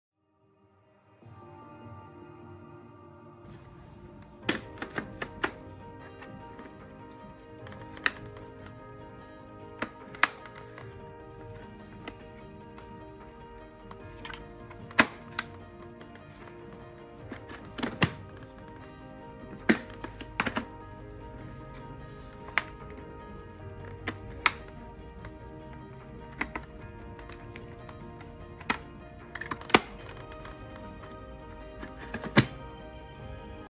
CABERG Droid bukósisak plexicsere
Hoztunk egy hasznos segédletet, ha plexit szeretnél cserélni a sisakodon! Széles Caberg bukósisak választékkal és a hozzájuk tartozó kiegészítőkkel, alkatrészekkel várunk benneteket Nagy Lajos király úti üzletünkben

📍 1149 Budapest, Nagy Lajos király útja 138. (a Bosnyák tér mellett)